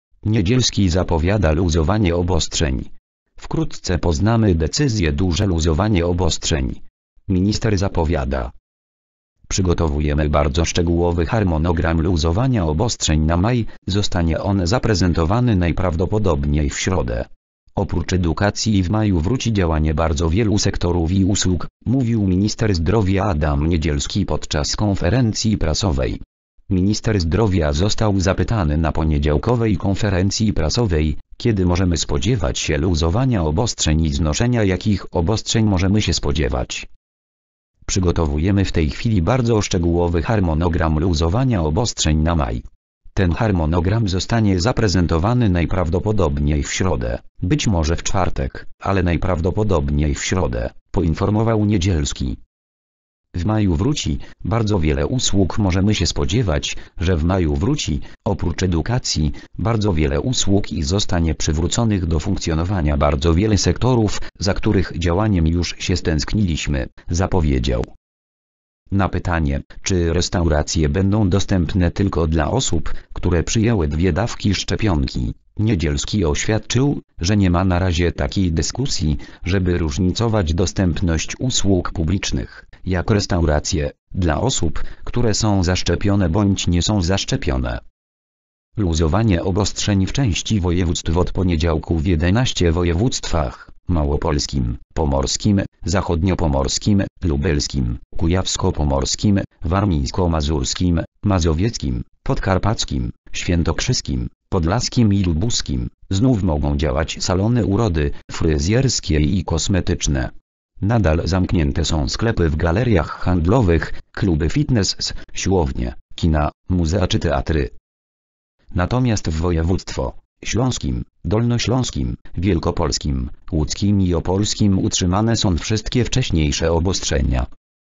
Niedzielski zapowiada luzowanie obostrzeń. Wkrótce poznamy decyzje.
Duże luzowanie obostrzeń. Minister zapowiada.

Jesli tylko masz ochote wspierac to co robie oraz nasz kanał, bardzo ci dziekuje.nr konta: IBAN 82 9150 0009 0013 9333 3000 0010
   (fot. Twitter.com/tvp.info - wideo zrzutu ekranu zdjęcie)